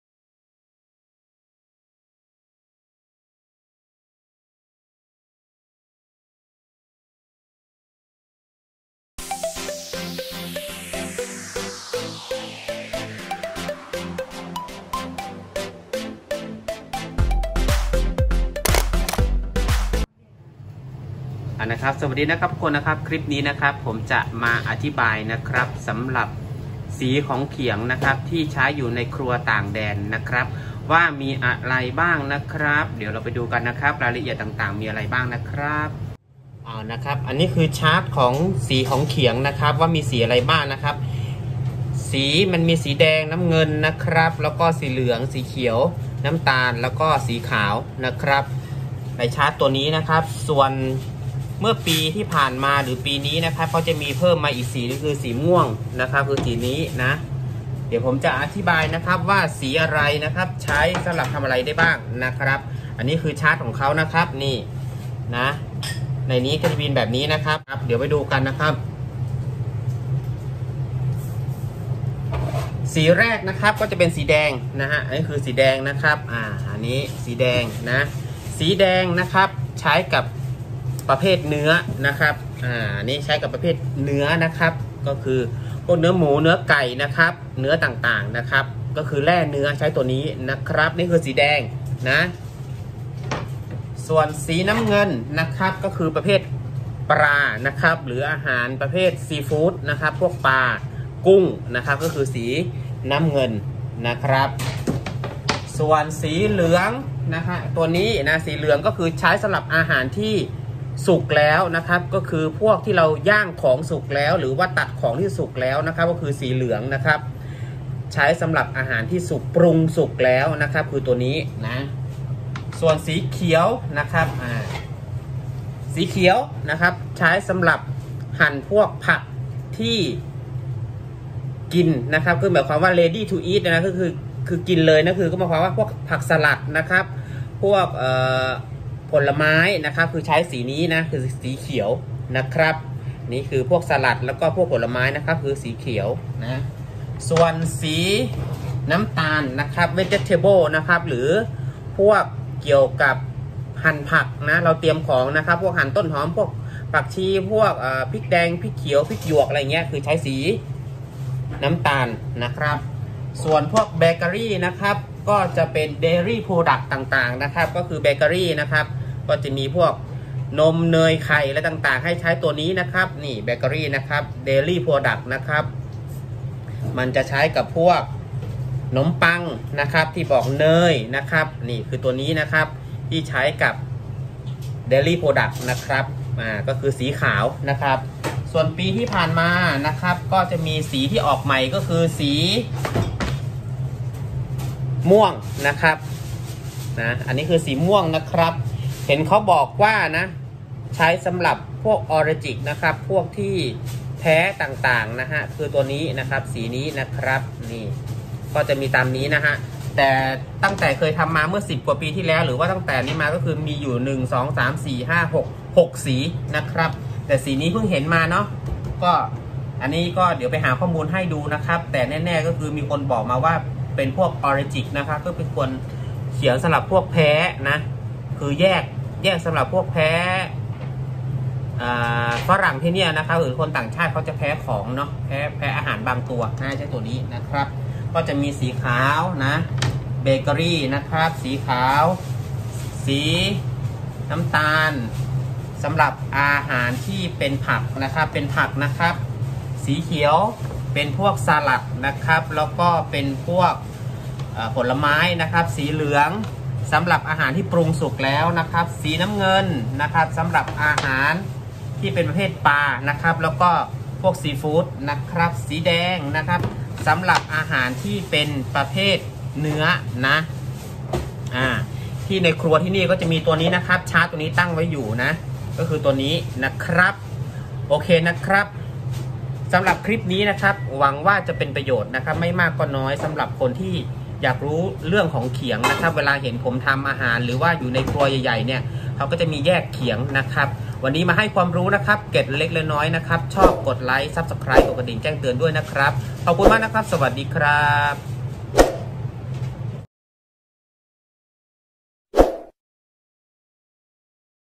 เรื่องของเขียง ครัวไทยในต่างแดน/chawalit mangkla
เรื่องของเขียง
สีแดง เนื้อ ของสด
สีเหลือง ของปรุงสุก
สีน้ำเงิน ปลา กุ้ง ซีฟู้ด
สีเขียว ผักสลัด ผลไม้
สีน้ำตาล ผักสดนำทำอาหาร
สีขาว เนย นม ไข่
สีม่วง สำหรับคนแพ้อาหาร